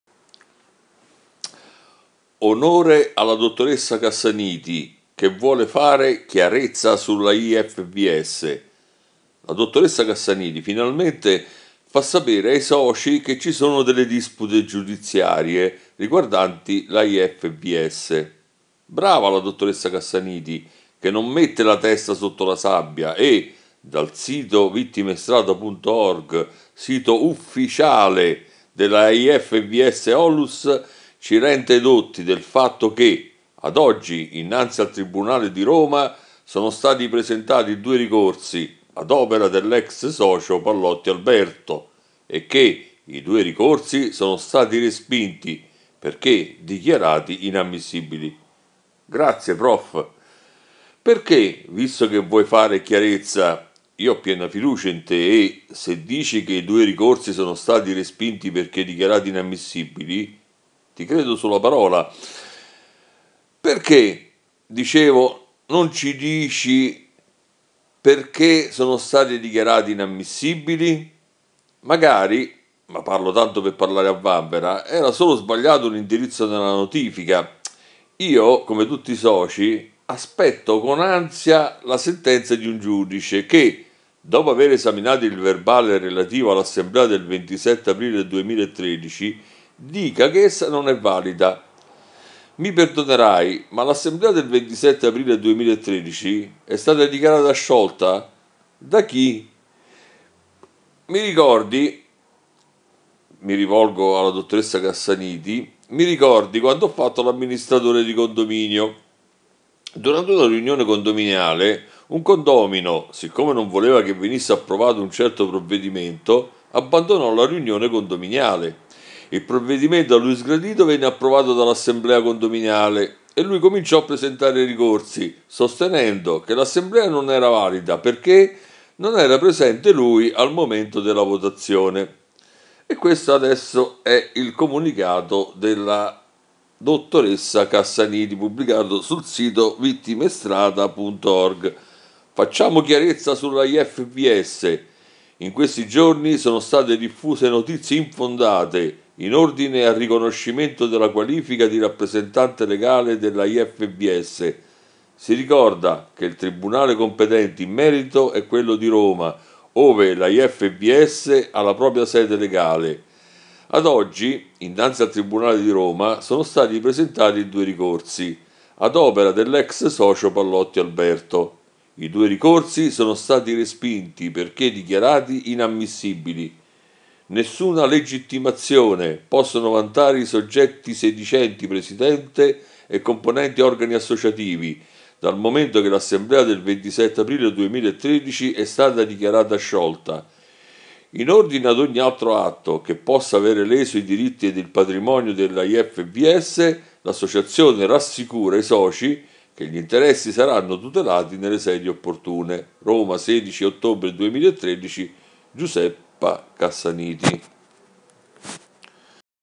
Cassaniti, Pallotti e le vertenze giudiziarie dell' AIFVS - onlus
Onore alla dottoressa Cassaniti, che vuole fare "chiarezza sull'AIFVS"!
La dottoressa Cassaniti, finalmente, fa sapere ai soci che ci sono delle dispute giudiziarie riguardanti l'AIFVS!
BRAVA la dottoressa Cassaniti, che non mette la testa sotto la sabbia e, dal sito vittimestrada.org, sito U F F I C I A L E dell'AIFVS-onlus, ci rende edotti del fatto che "ad oggi innanzi al tribunale di Roma sono stati presentati due ricorsi ad opera dell'ex socio Pallotti Alberto" e che "i due ricorsi sono stati respinti perché dichiarati inammissibili.".
Grazie, prof!
Perché, visto che vuoi fare "chiarezza" (io ho piena fiducia in te e, se dici che "i due ricorsi sono stati respinti perché dichiarati inammissibili", ti credo sulla parola), non ci dici perché sono stati dichiarati "inammissibili"?
Magari, ma parlo tanto per parlare a vanvera, era solo sbagliato un indirizzo nella notifica...
Io, come tutti i soci, aspetto con ansia la sentenza di un giudice che, dopo aver esaminato il verbale relativo all'assemblea del 27 aprile 2013, dica che essa non è valida: mi perdonerai, ma l'assemblea del 27 aprile 2013 è stata dichiarata sciolta... da chi?
Mi ricordi quando ho fatto l'amministratore di condominio: durante una riunione condominiale, un condomino, siccome non voleva che venisse approvato un certo provvedimento, abbandonò la riunione condominiale; il provvedimento a lui sgradito venne approvato dall'assemblea condominiale e lui cominciò a presentare ricorsi, sostenendo che l'assemblea non era valida... perché... non era presente lui al momento della votazione...

Facciamo chiarezza sull'AIFVS
In questi giorni sono state diffuse notizie infondate in ordine al riconoscimento della qualifica di rappresentante legale dell'AIFVS.
Si ricorda che il tribunale competente in merito è quello di Roma, ove l'AIFVS ha la propria sede legale.
Ad oggi innanzi al tribunale di Roma sono stati presentati due ricorsi ad opera dell'ex socio Pallotti Alberto.
I due ricorsi sono stati respinti perché dichiarati inammissibili.
Nessuna legittimazione possono vantare i soggetti sedicenti presidente e componenti organi associativi, dal momento che l'assemblea del 27 aprile 2013 è stata dichiarata sciolta.
In ordine ad ogni altro atto che possa avere leso i diritti ed il patrimonio dell'AIFVS, l'Associazione rassicura i soci che gli interessi saranno tutelati nelle sedi opportune.
Roma 16/10/2013
Giuseppa Cassaniti.

Pagina personale di Claudio Martino. Non riporta le posizioni ufficiali dell' AIFVS ( a.i.f.v.s. ) onlus né di altra associazione.